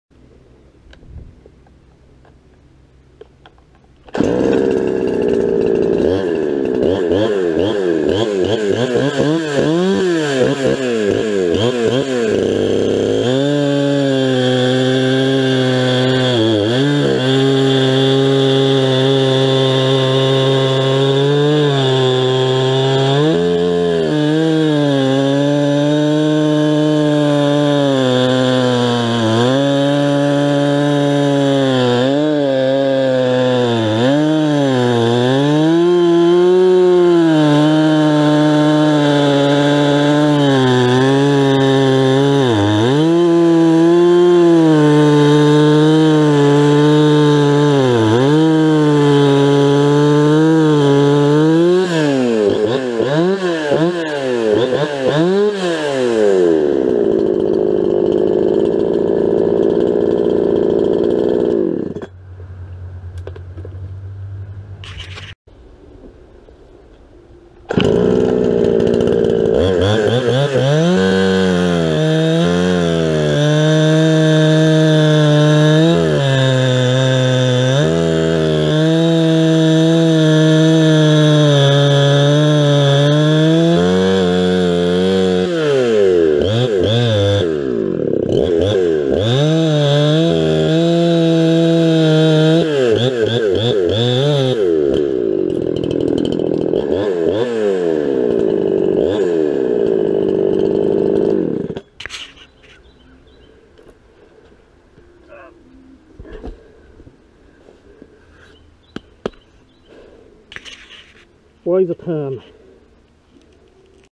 Farmertec Huztl 372xp kit Chainsaw Cutting seasoned HardWood Hand Filed Chain
cutting up some firewood I had laying around with my ported Huztl 372..running 20" full comp hand filed chain...this is really hard seasoned oak. Saw has a pretty aggressive wood's port...Saw was built using both huztl and oem parts...